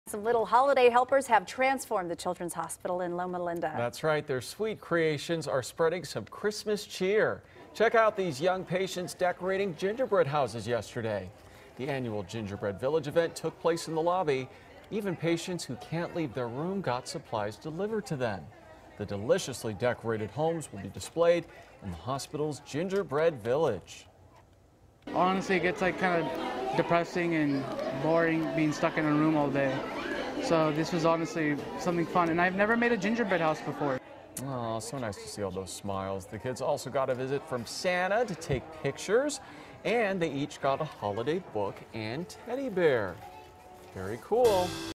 Young Patients Try Their Hands At Gingerbread House Decorating
One teenage patient said he had never made a gingerbread house before. Suzanne Marques and Peter Daut report.